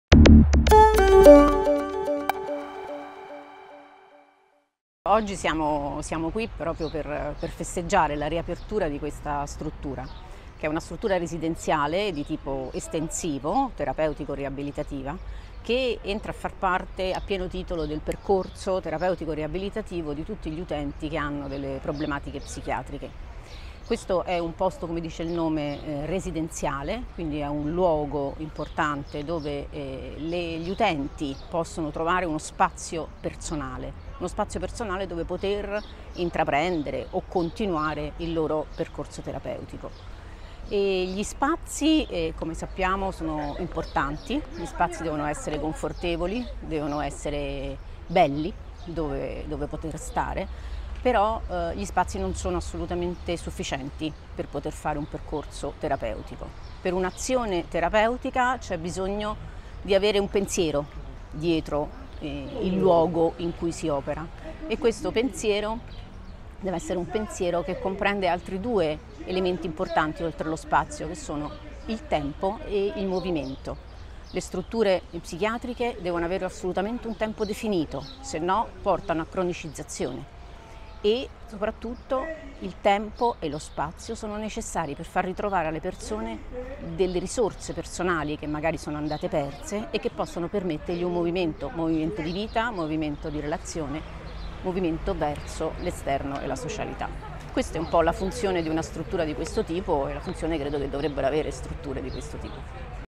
La riapertura della SRTRe Marcigliana: intervista alla Dott.ssa Marcella Fagioli
La Dottoressa Marcella Fagioli, Responsabile della SRTRe Marcigliana, ci parla della riapertura della struttura e del significato delle terapie riabilitative per le vite delle persone prese in carico dal Centro.